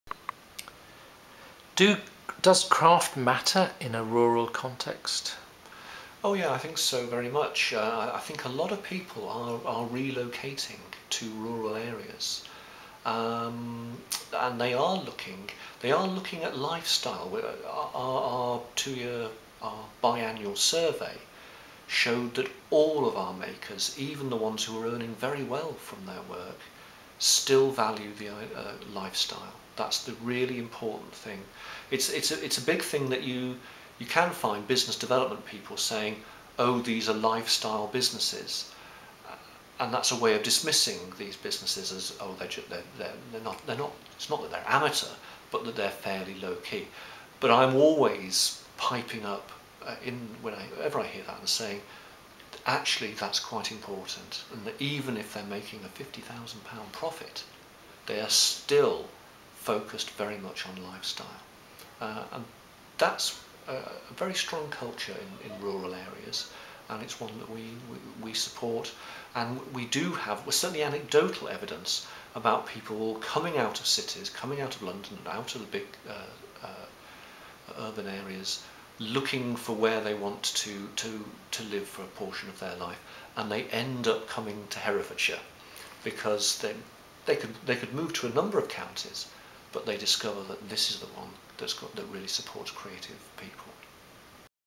Andy Dawson importance of craft in rural context 9
Andy Dawson, Creative Industries Co Ordinator within Herefordshire County Council talking to Michael Noonan about the importance of craft within Herefordshire.

This interview took place Friday 11th March 2011 in Chalk Farm, London.